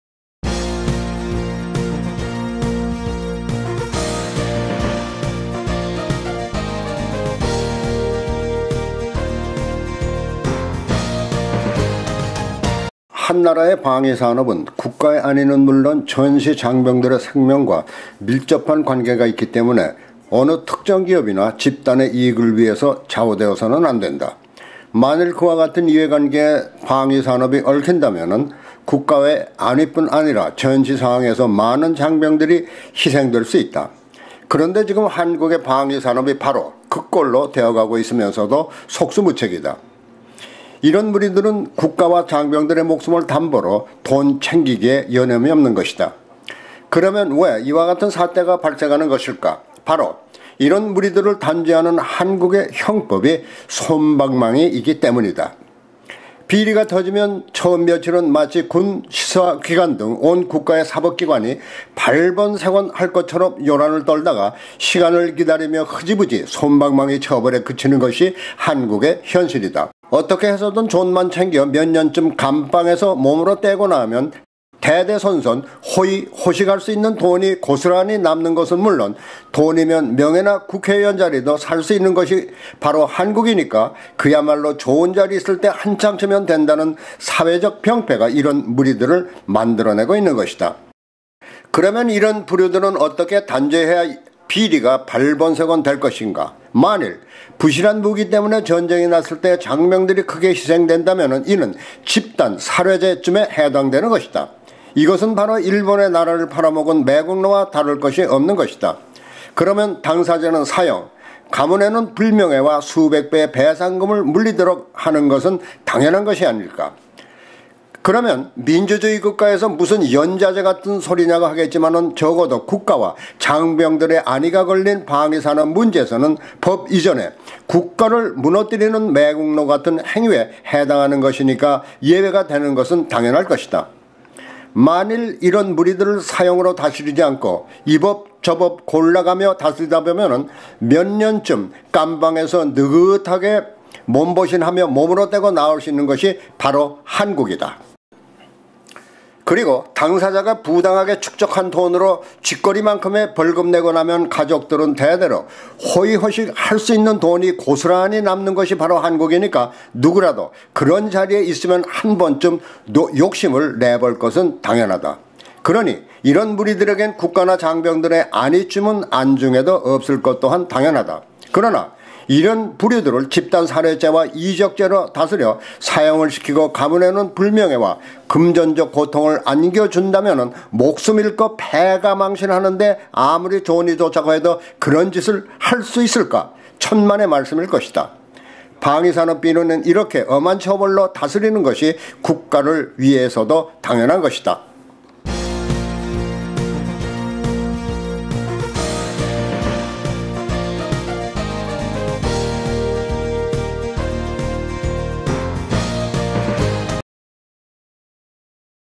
방산비리는 철퇴로 다스려야
한국은 어떤 비리에도 솜방망이 처벌로 다스리는 사법관행 때문에 국가적 위기를 초래할수 있는  방위산업 비리 같은 범죄를 쉽게 저지를수 있었다